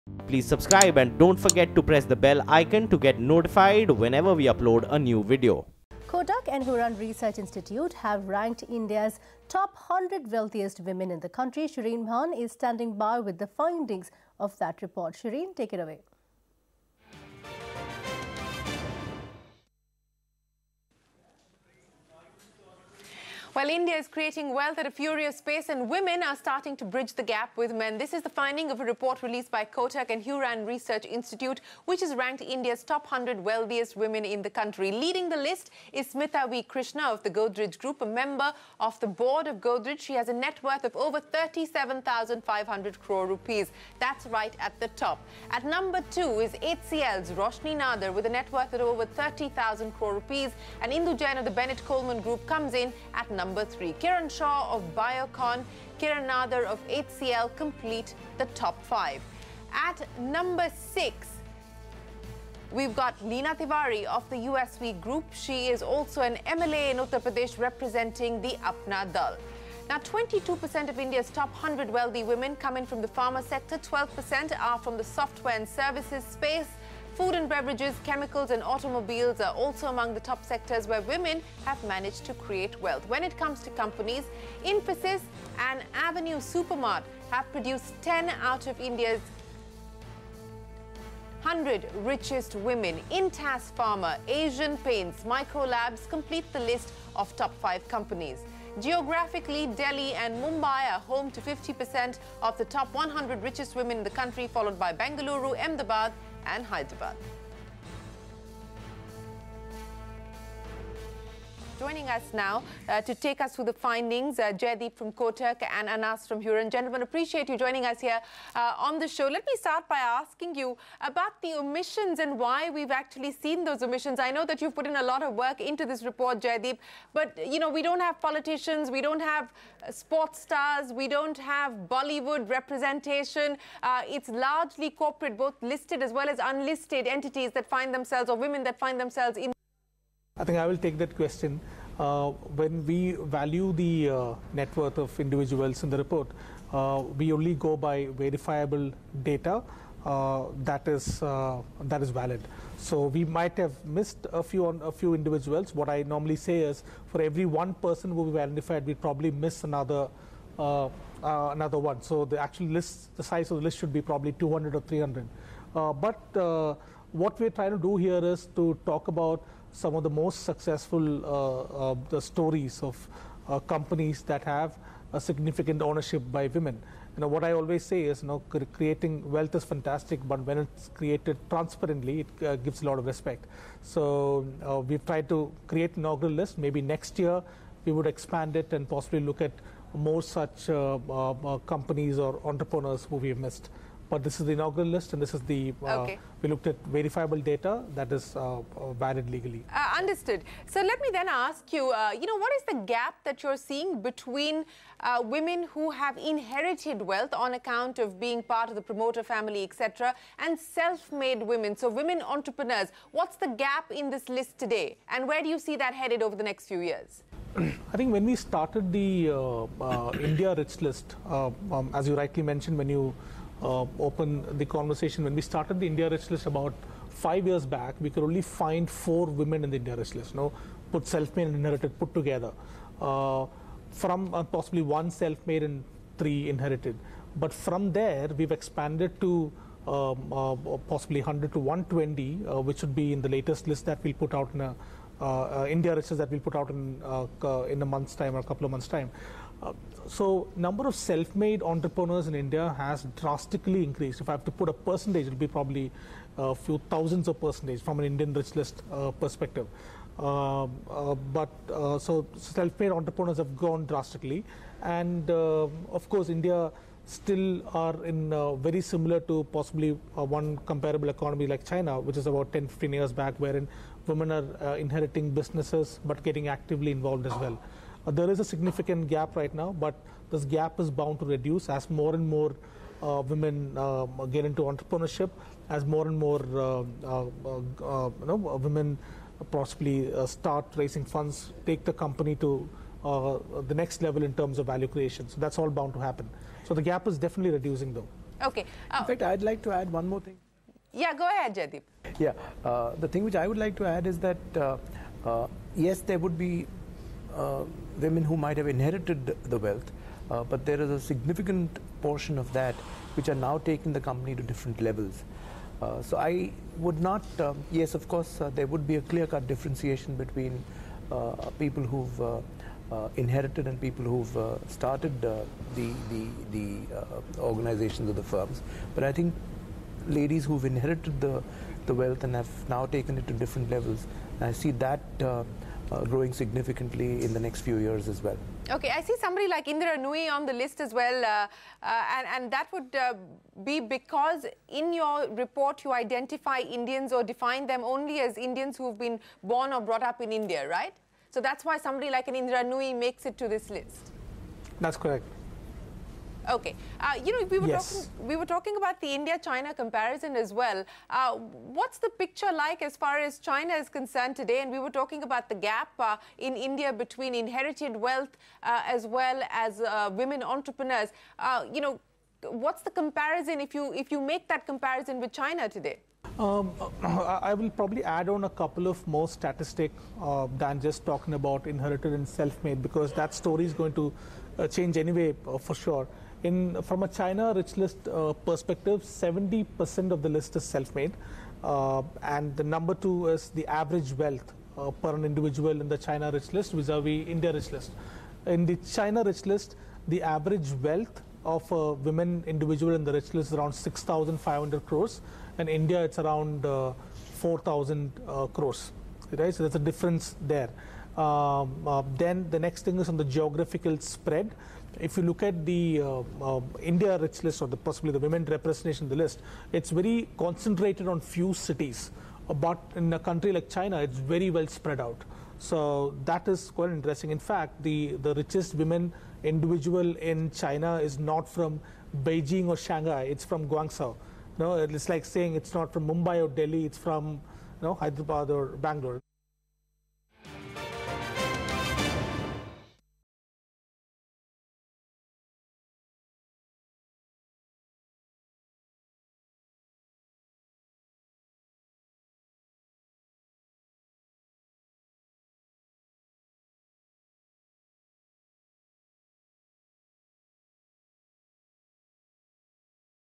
Smitha V Crishna, of Godrej Tops the 100 Wealthiest Women in India List: Kotak Wealth Hurun Report
Kotak Wealth Hurun Women 2018: Smitha V Crishna, of Godrej has topped the list in Kotak Wealth Hurun - Leading Wealthy Women 2018 with a net worth of Rs 37,570 crore. We take a look at top 10 names.